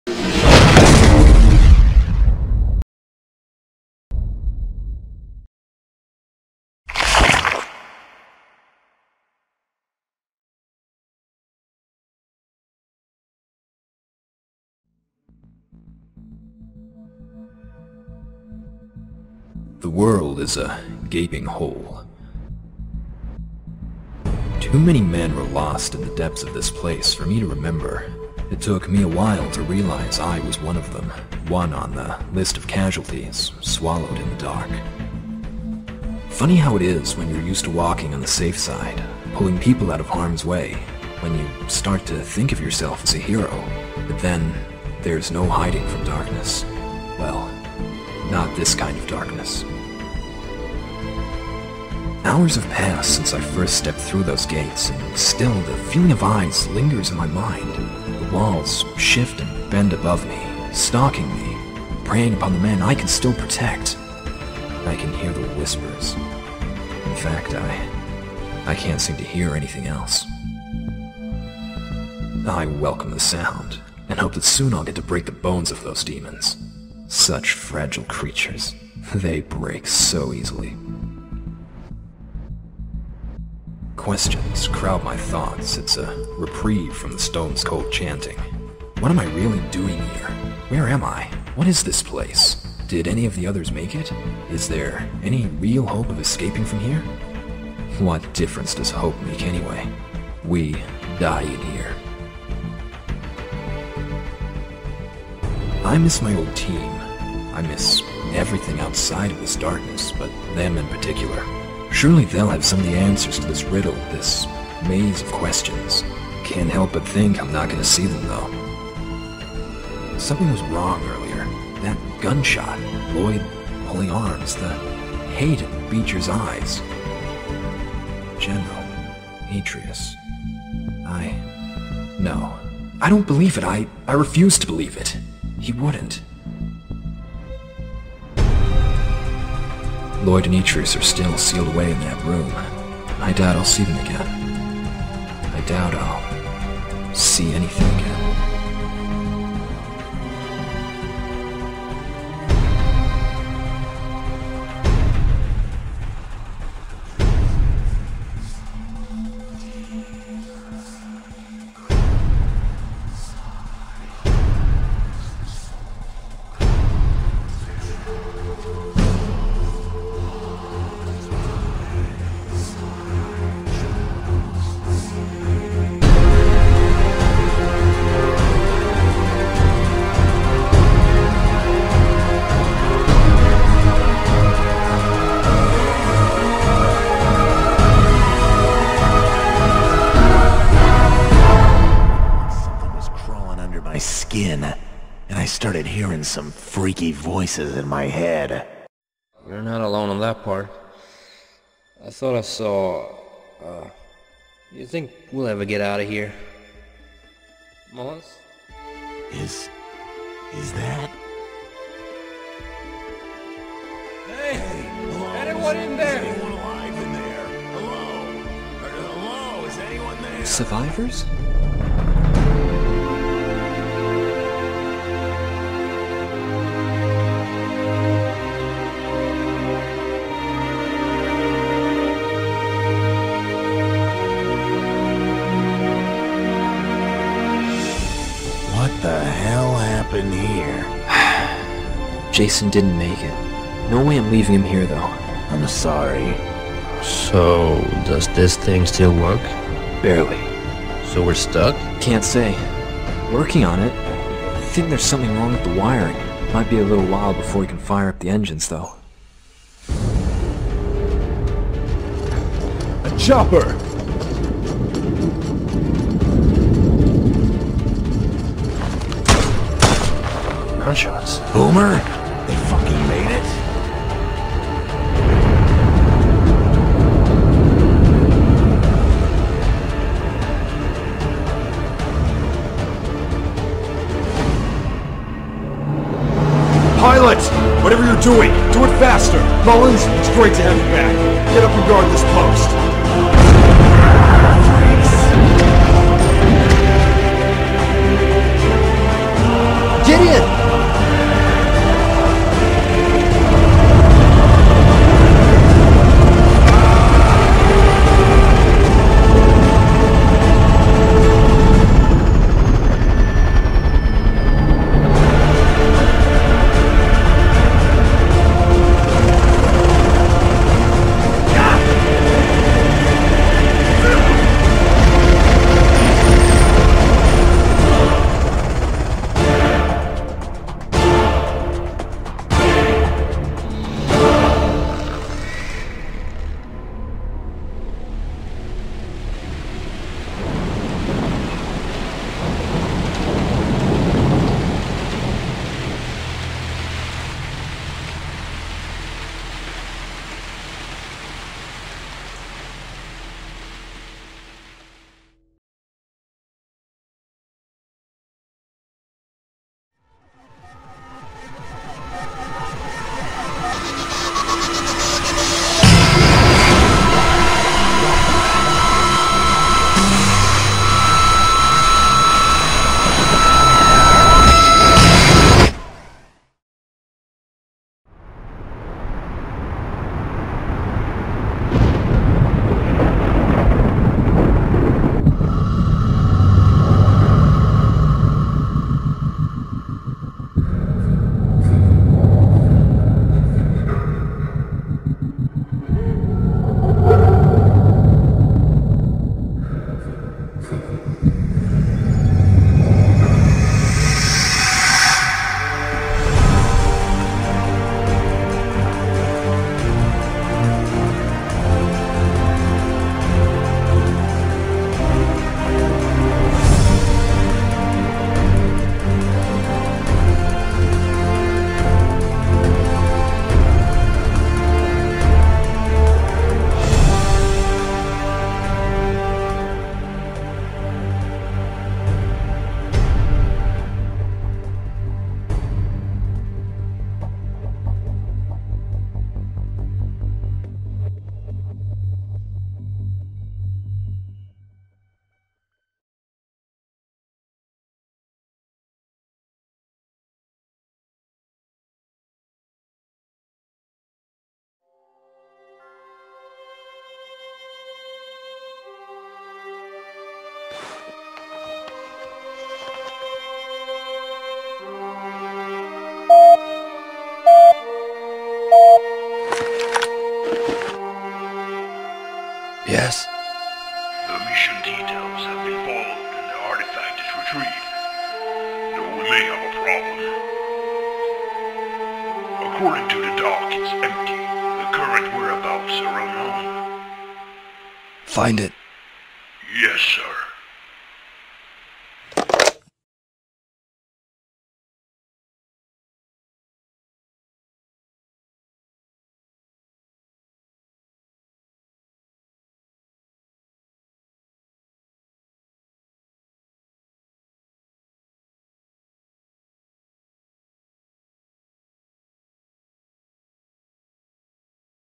Castle Series - Castle Repercussions Part D2 (1080p)
Official upload in 1080p! In Castle Repercussions, we focus on the crew-memebers who were left behind after the events of Castle III.

Part D. Finally, Elite Team Demolitions Expert "Boomer" - The strong and brutal leader.

For the ultimate source of Castle Series information and updates, find me and the series' progress